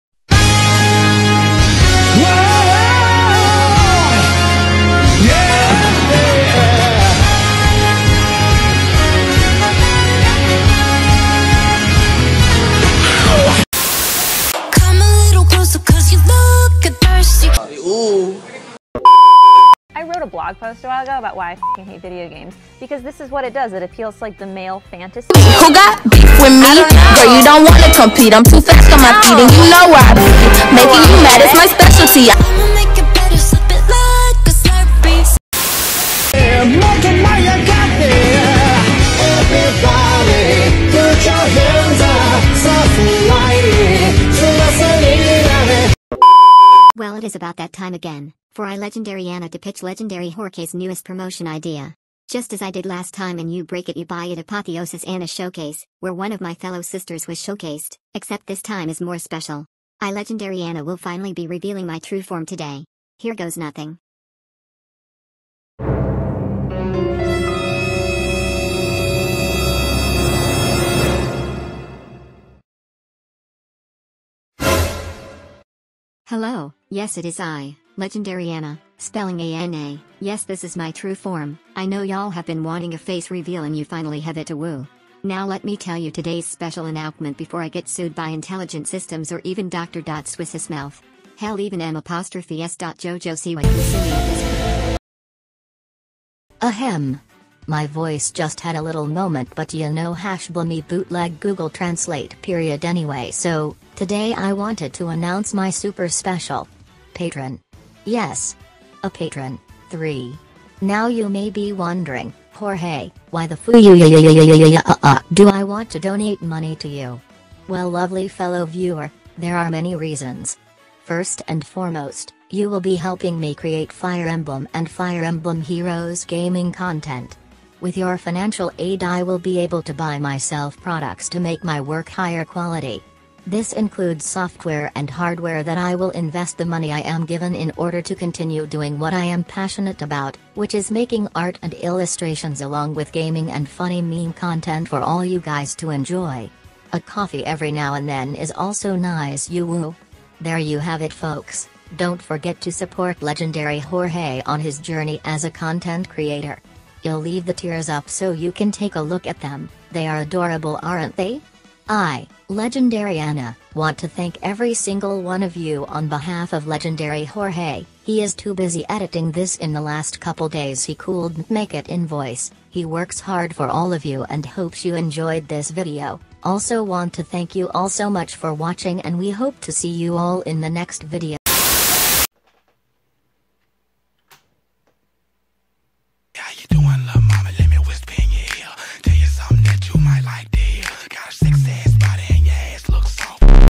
Legendary Corrin ( ͡° ͜ʖ ͡°)
Legendary Corrin sure is something. ( ͡° ͜ʖ ͡°)

Basically my Legendary Corrin Reaction, can't wait to make my Legendary Corrin Summoning!

_Fallen Corrin has left the chat_